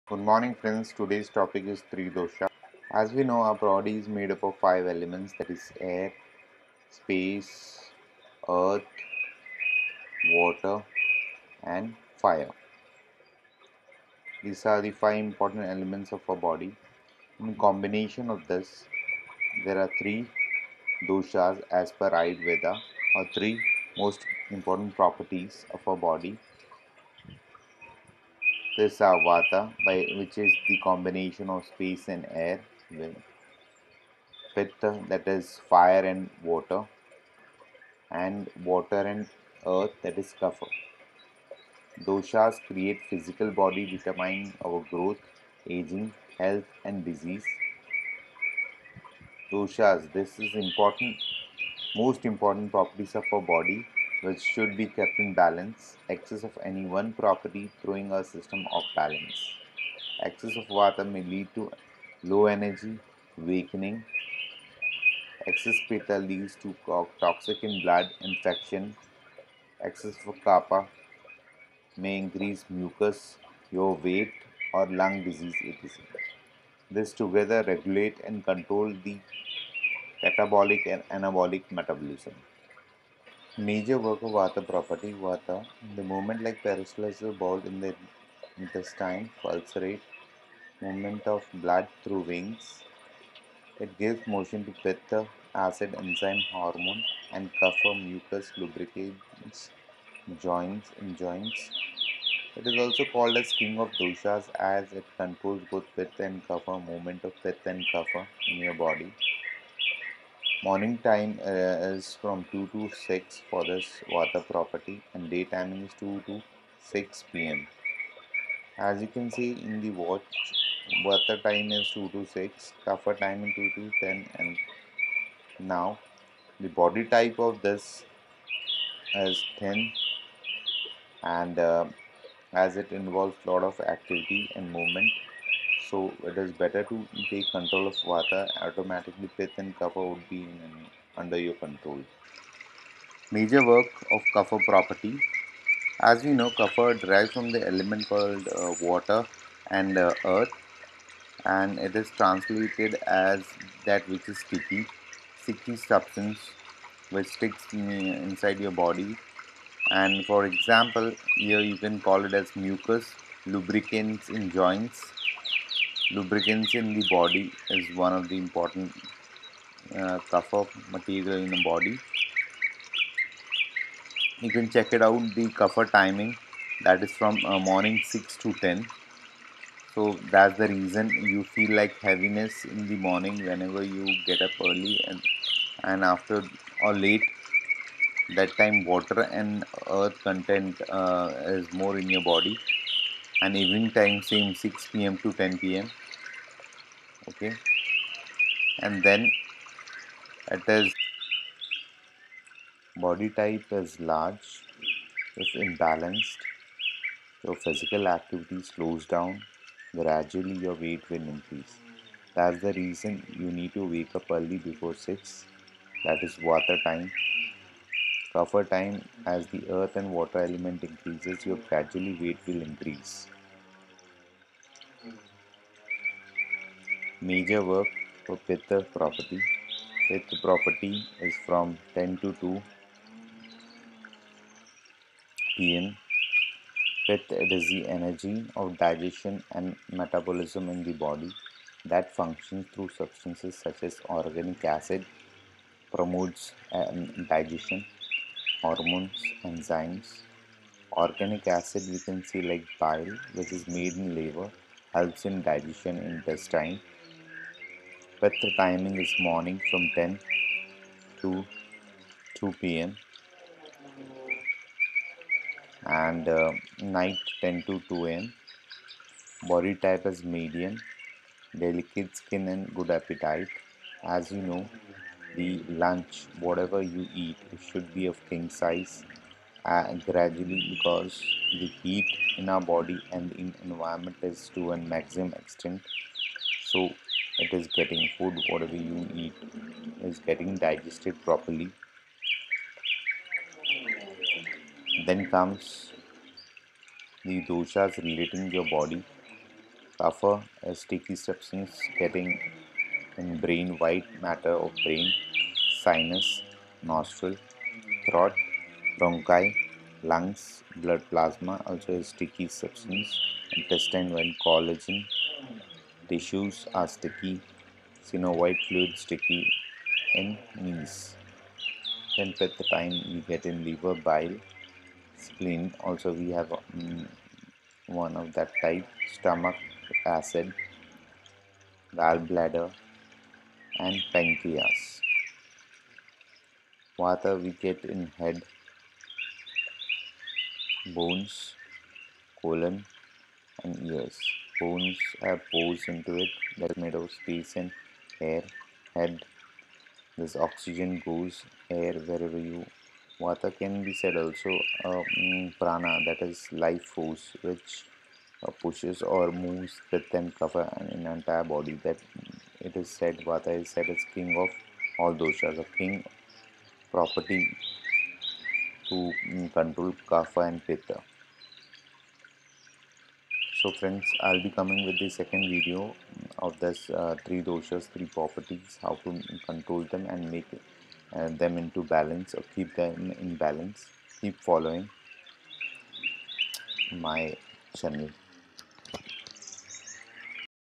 3 Dosha Vata pitta kapha
3 Dosha major property of our body made of five element fire water air space earth.
Can target yourself for your weight management i.e
weight lose
weight gain